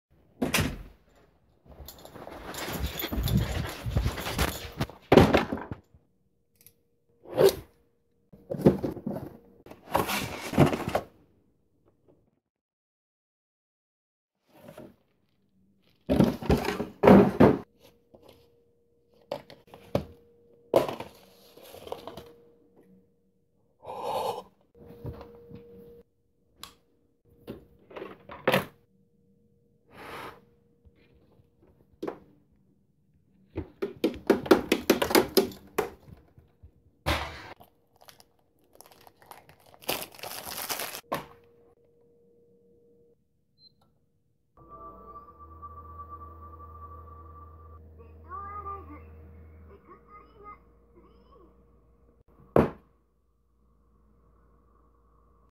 A System Seller
My Dead or Alive Xtreme 3 (And other trash) arrived in muh mail today.

Dead or Alive Xtreme 3 is definitely the only reason Playstation 4 is still selling. ( ͡° ͜ʖ ͡°)